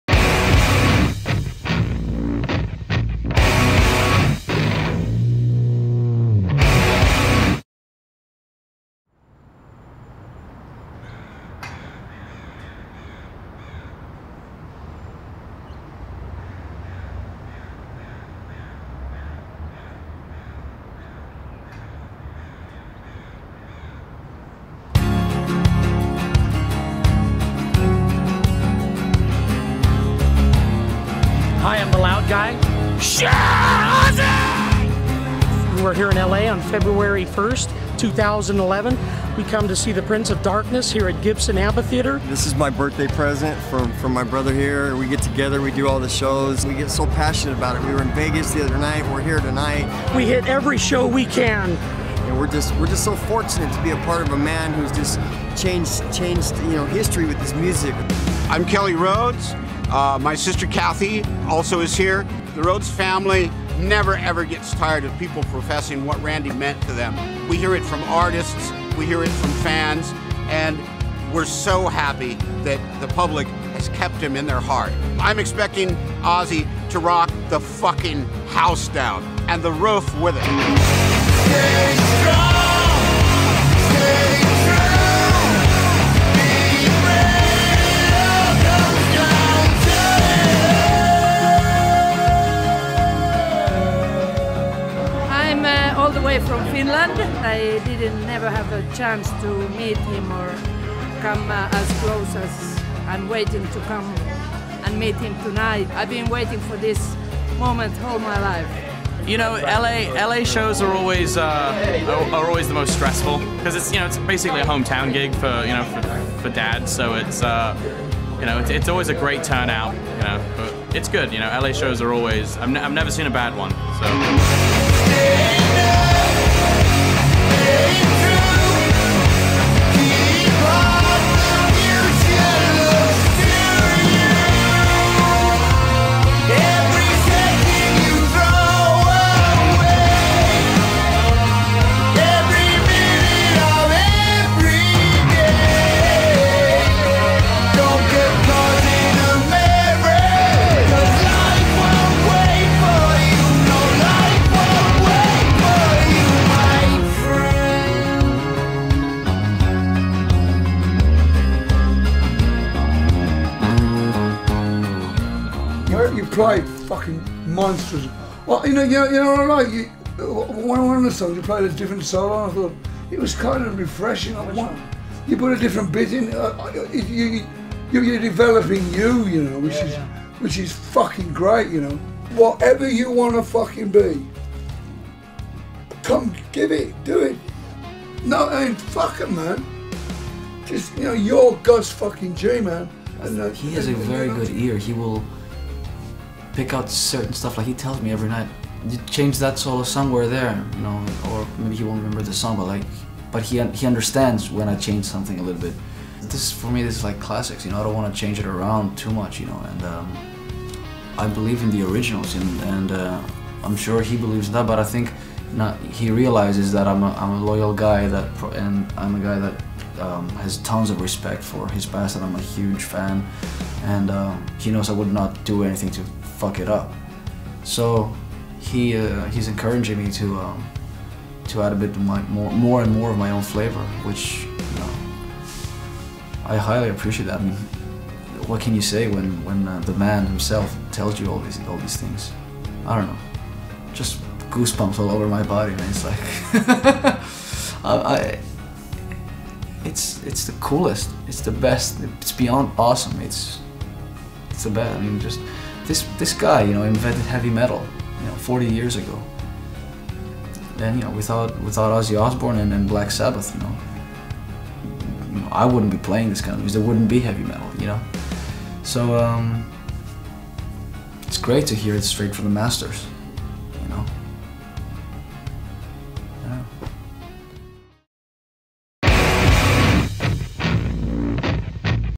OzTV - Los Angeles Feb 1, 2011
OzTV checks in with Ozzy & the guys at their stop in Los Angeles at the Gibson Ampitheatre on Feb 1, 2011